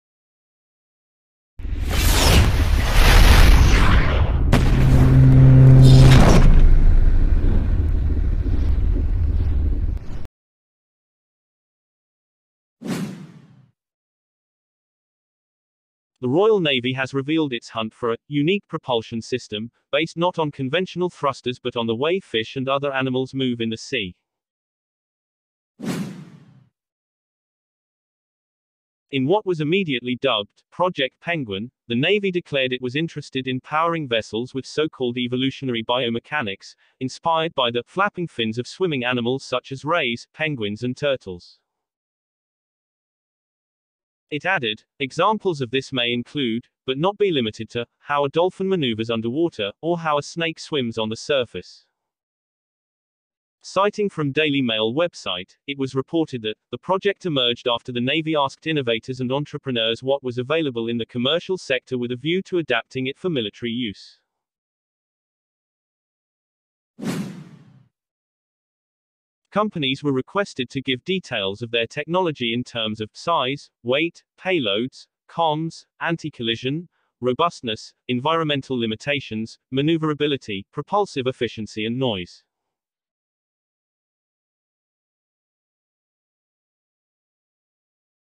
WOW! Royal Navy to put fins on its submarines to propel ship dubbed 'Project Penguin'
Royal Navy to put fins on its submarines to propel ship dubbed 'Project Penguin'

The Royal Navy has revealed its hunt for a ‘unique propulsion system’ – based not on conventional thrusters but on the way fish and other animals move in the sea.

In what was immediately dubbed ‘Project Penguin’, the Navy declared it was interested in powering vessels with so-called evolutionary biomechanics, inspired by the ‘flapping fins of swimming animals such as rays, penguins and turtles’.

Warning!!!